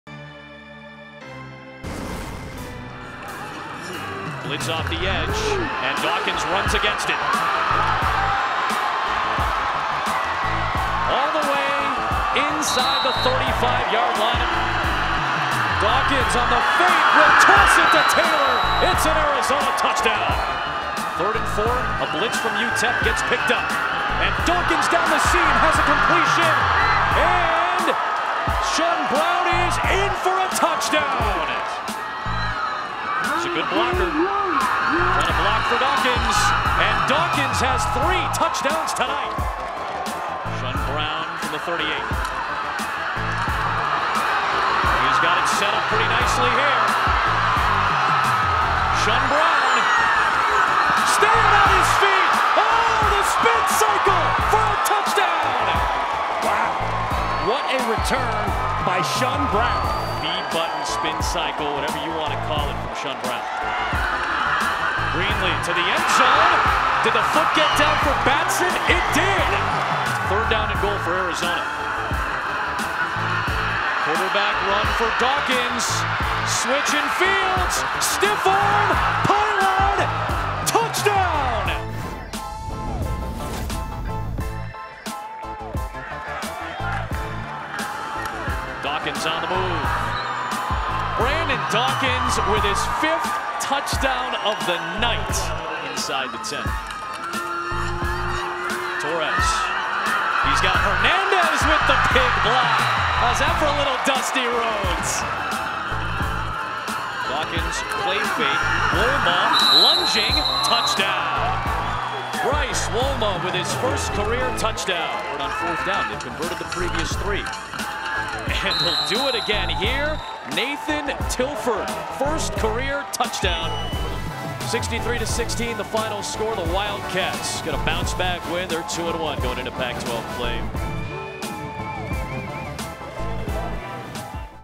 Highlights: Brandon Dawkins' career night helps Arizona football steamroll UTEP
Watch as Arizona football defeated UTEP, 63-16. Wildcats' QB  Brandon Dawkins' three rushing TDs ties for a career high, and his six TDs overall & three passing TDs are both new career highs.